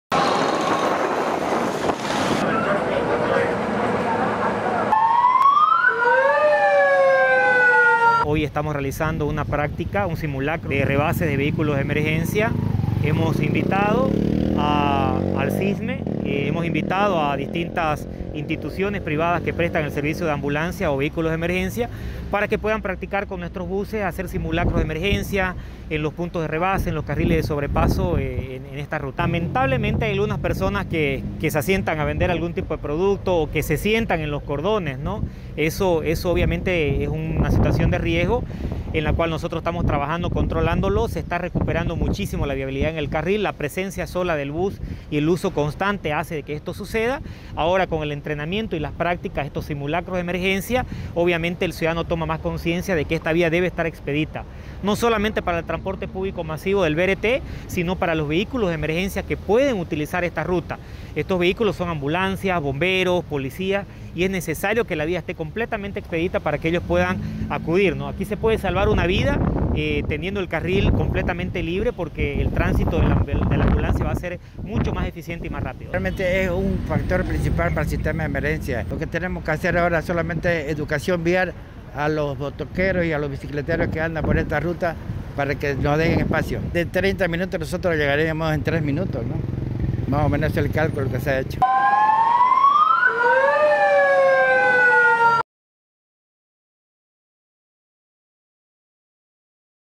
Municipio realizó simulacro de emergencia en la vía del BRT y harán control para dejar completamente
Municipio realizó simulacro de emergencia en la vía del BRT y harán control para dejar completamente libre